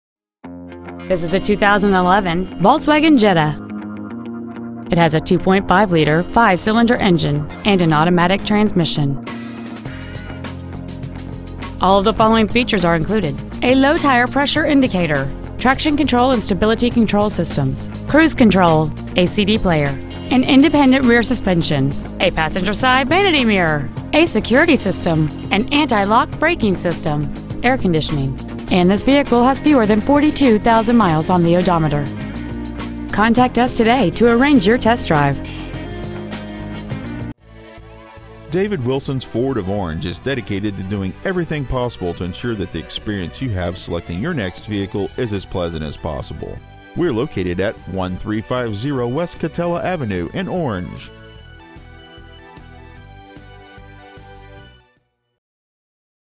Used 2011 VOLKSWAGEN JETTA Orange CA
Year : 2011
 Make : VOLKSWAGEN
 Model : JETTA
 Engine : 2.5L I5 20V MPFI DOHC
 Trans . : 6-SPEED AUTOMATIC
 Exterior : CANDY WHITE
 Miles : 41,708
 Interior : TITAN BLACK
 Stock : 765862

 1350 W. Katella